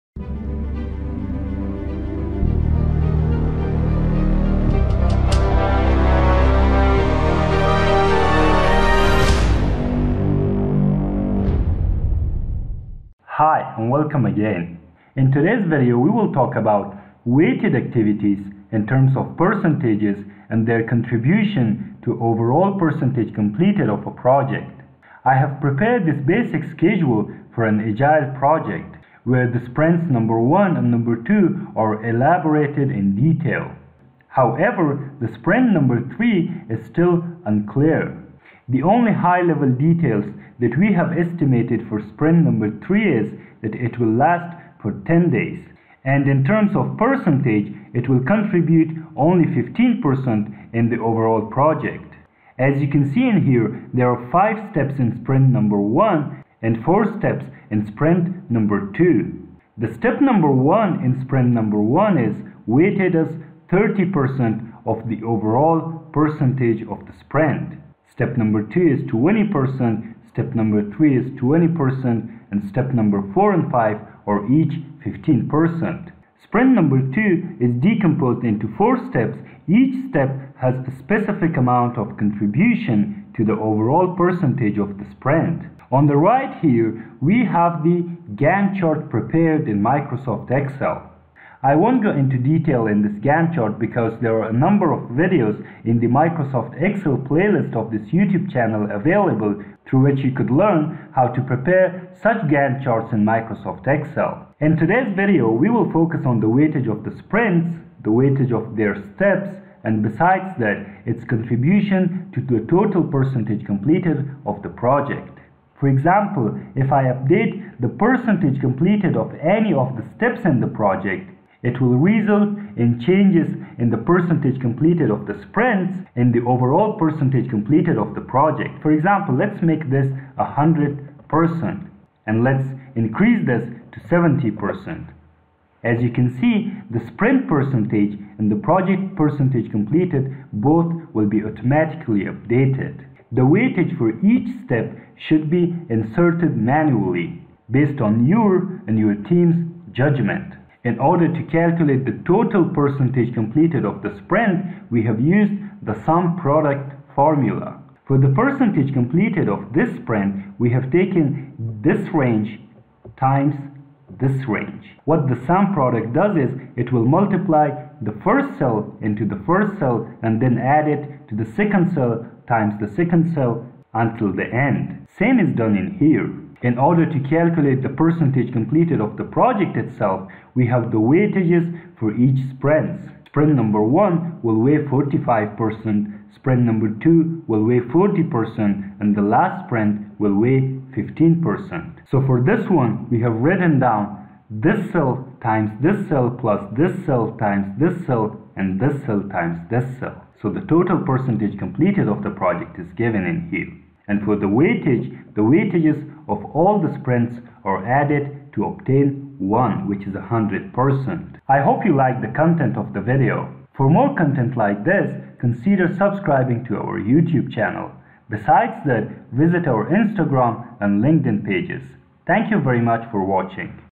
How to Calculate Project Percentage Completed from Weighted Activities in an Agile Project?
You can watch and learn how to prepare Gantt charts in Ms. Excel using the following links: